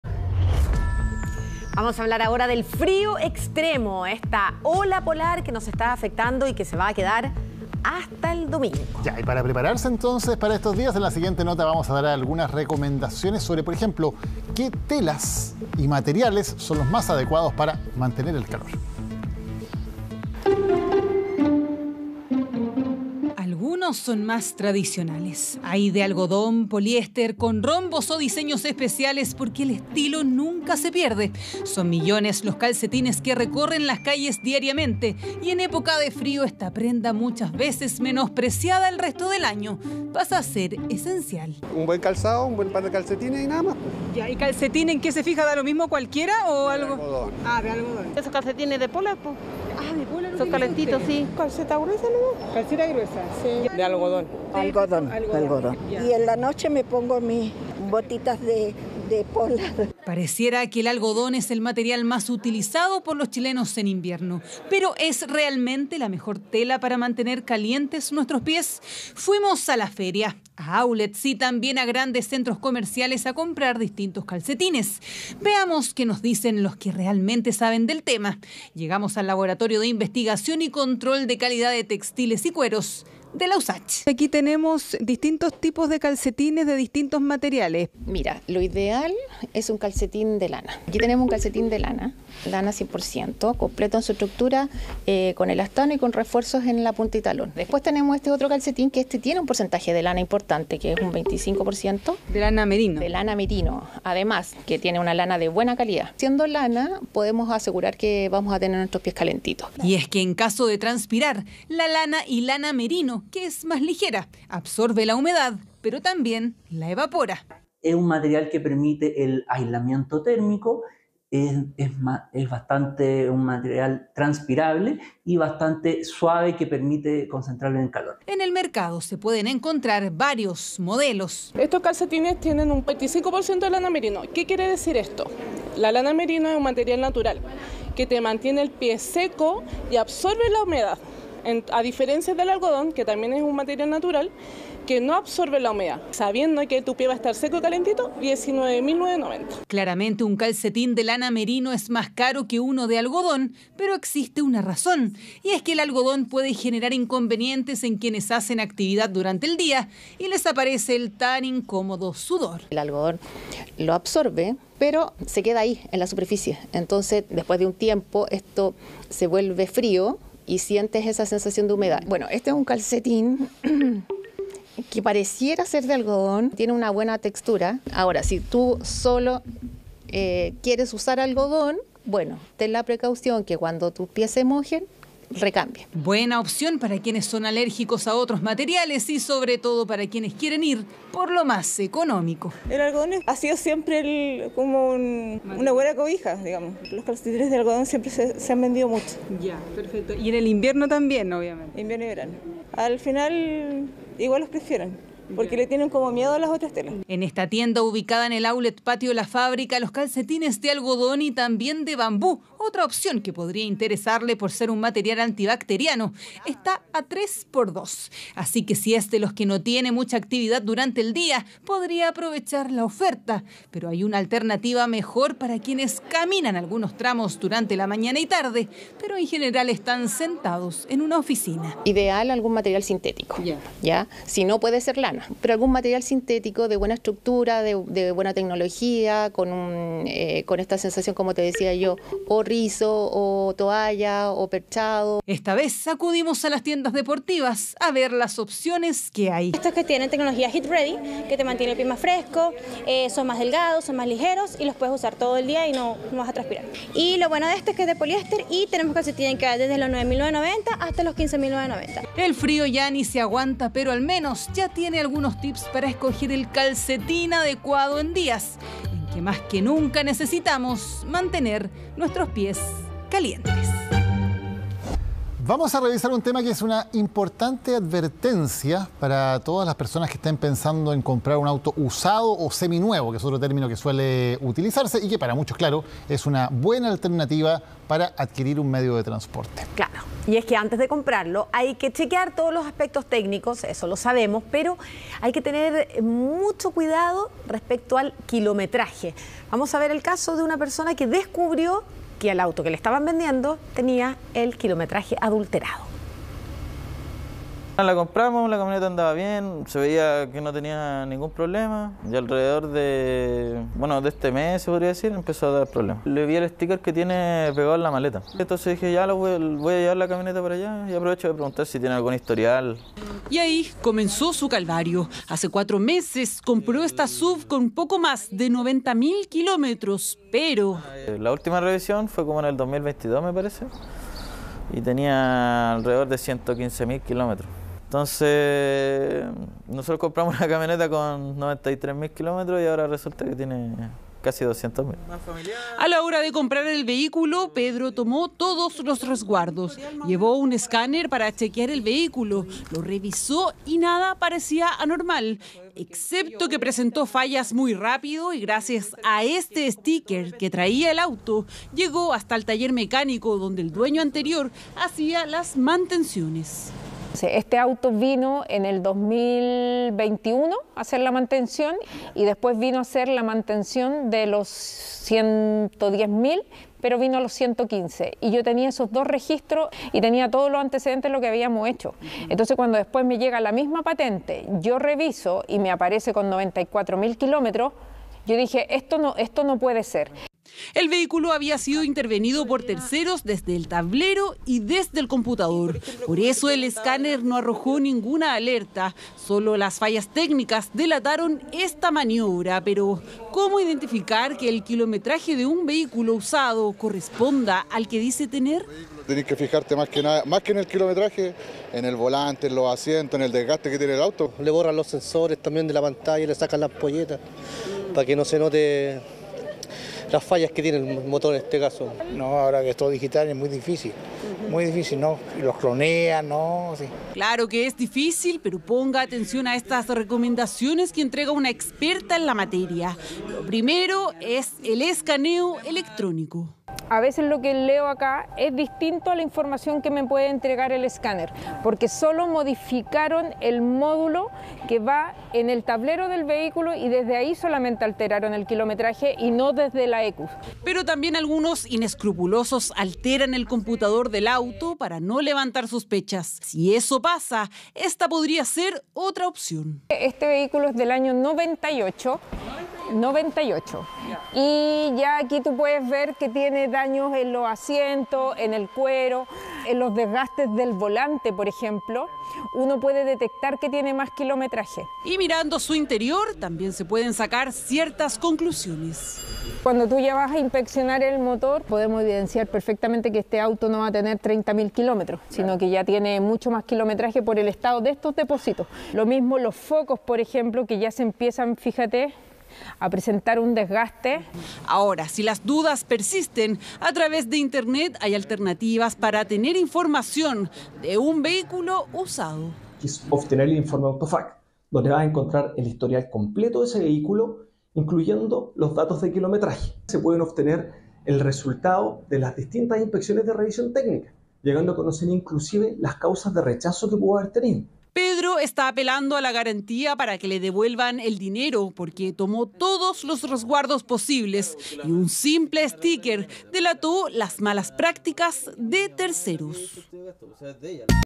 PONEMOS A PRUEBA: ¿Cuáles son los mejores calcetines para el frío? - CHV Noticias
En medio de días de ola polar, te entregamos algunas recomendaciones sobre las telas y materiales más adecuados para mantener el calor.